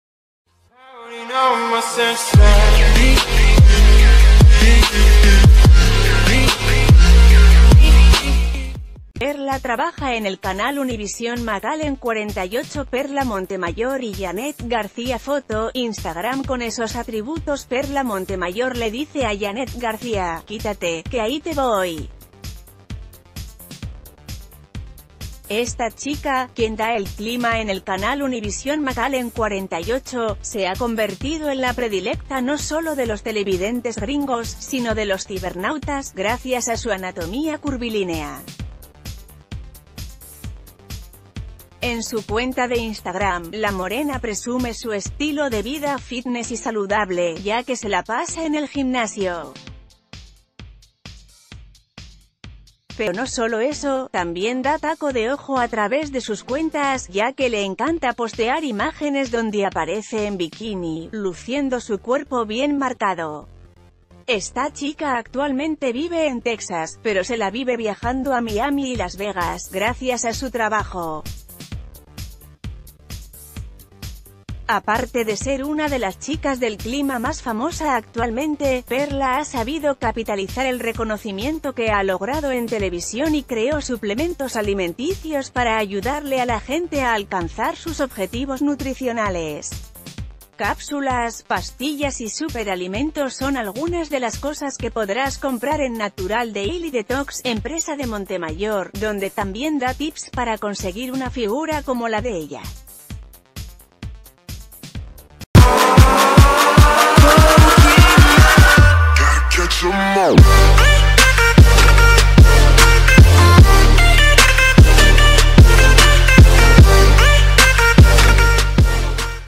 Perla Montemayor, la otra chica del clima que le hace “competencia” a Yanet García
Perla trabaja en el canal Univisión McAllen 48 Perla Montemayor y Yanet García Foto: Instagram Con esos atributos Perla Montemayor le dice a Yanet García “¡Quítate, que ahí te voy!”. Esta chica, quien da el clima en el canal Univisión McAllen 48, se ha convertido en la predilecta no sólo de los tele...